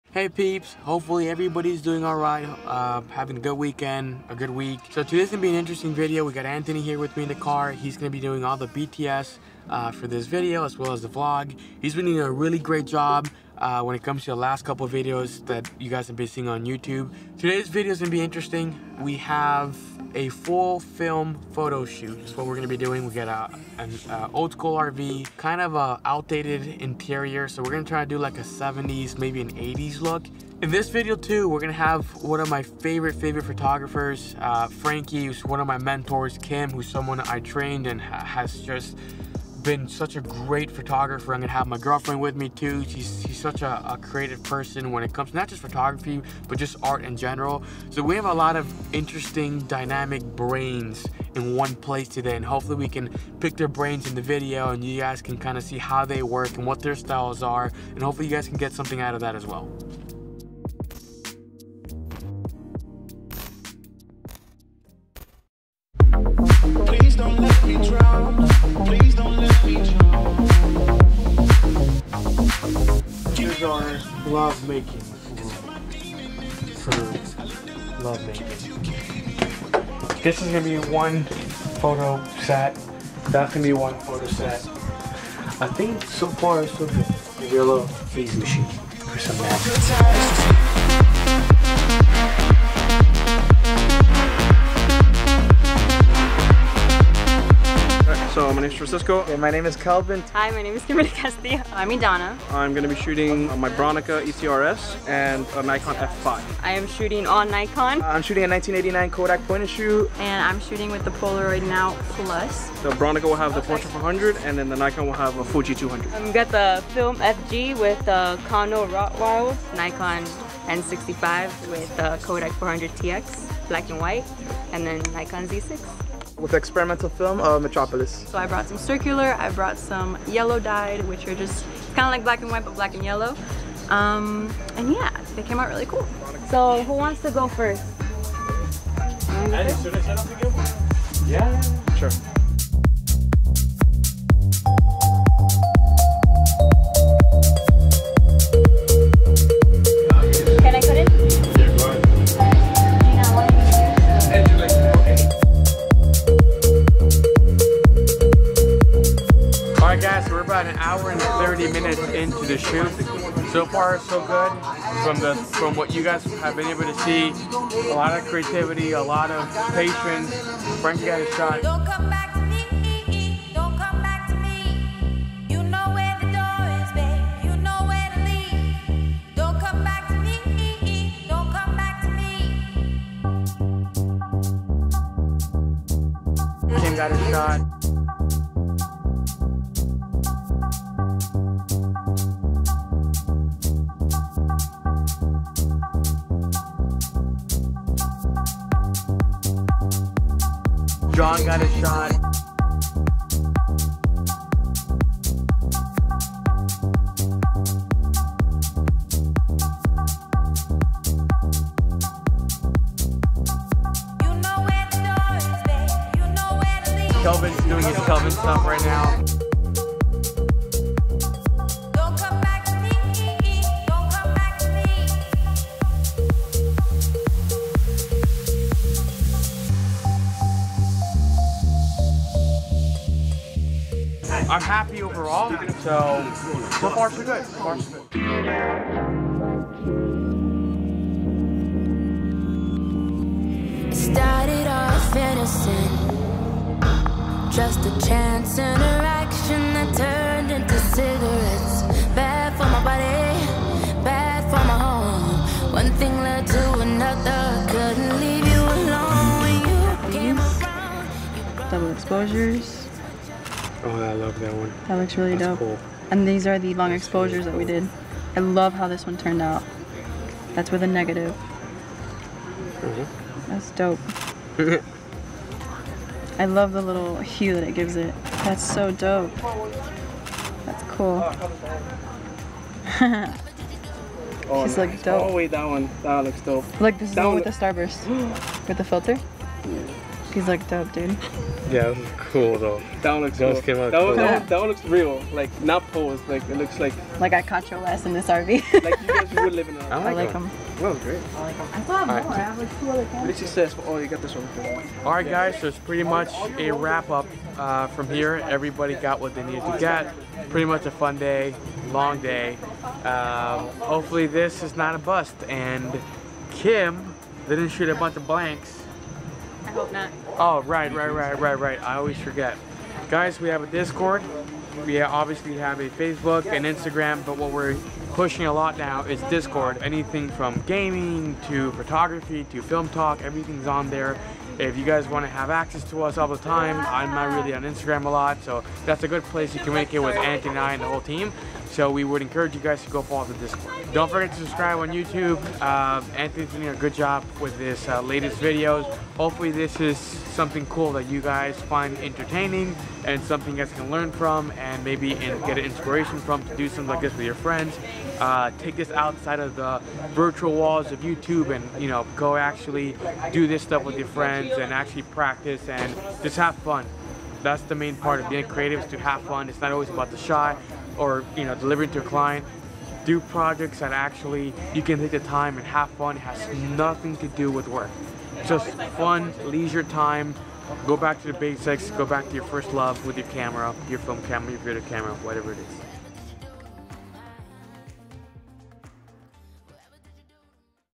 Young Wild Dreams & Friends - Old-School RV Film Shoot
Hey everyone! Today, Anthony and I went on a spontaneous adventure. We love it when our friends call us out of the blue to create or film something, and that's exactly what we did in this video.

We received a call from our friend Kim, who invited us to take photos of her model friends in her dad's RV. It was the last day before the RV was being sold, so we didn't want to miss out on the opportunity.